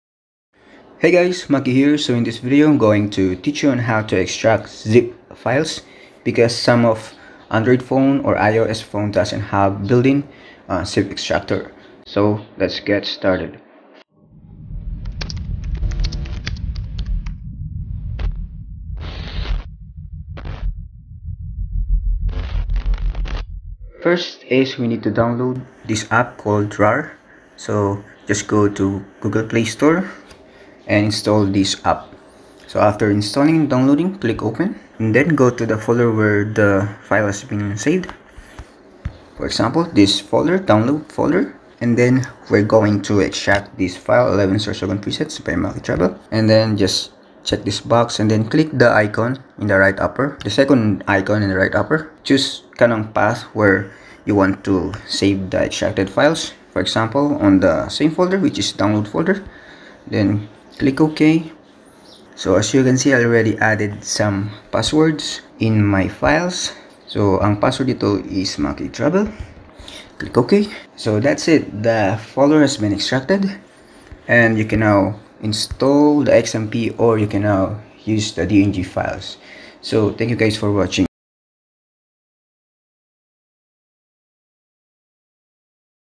How to Extract .zip Files | Macky Travel
Because i'm going to add password to my future presets i created this tutorial to prevent asking on how to extract and etc.